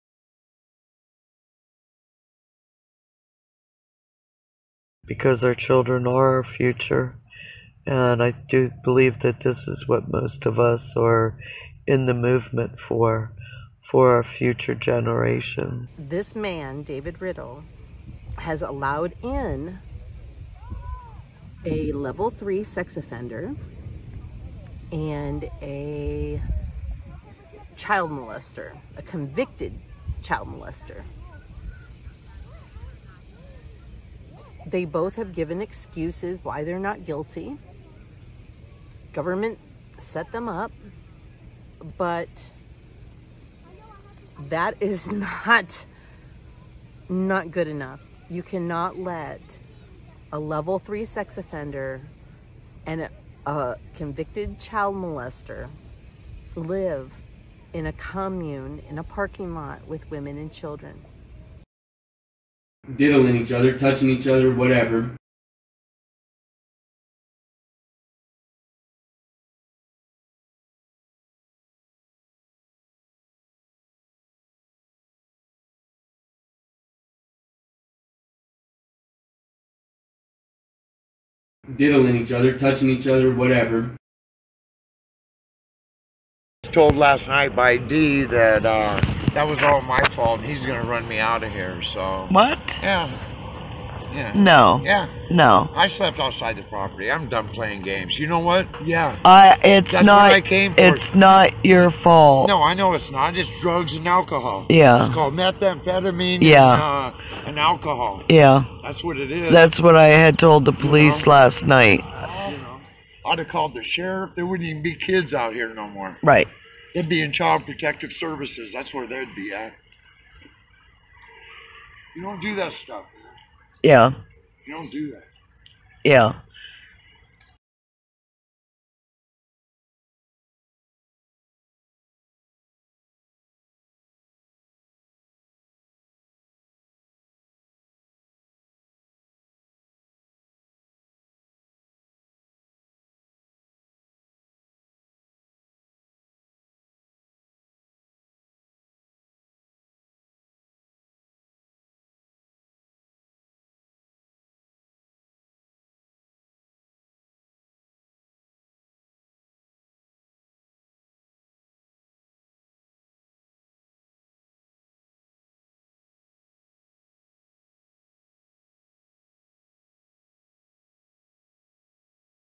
1776 Restoration Movement Sex Offenders, Pedophiles, Child Abusers RUN WILD at camp David Riddell
Shootings, Sucker Punches, Sex Offenders, Pedophiles, Child Abusers, Meth, seems to only be the tip of the iceberg!